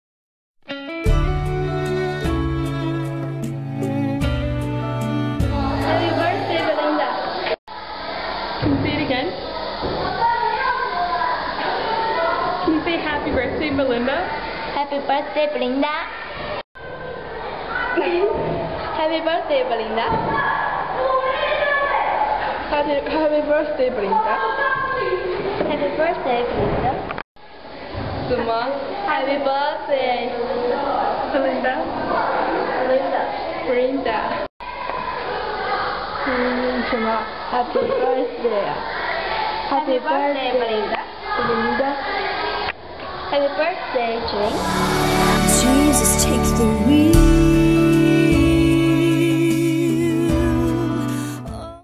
Happy Birthday, Belinda!
A birthday message from Chinese English students. Their teachers really like Belinda!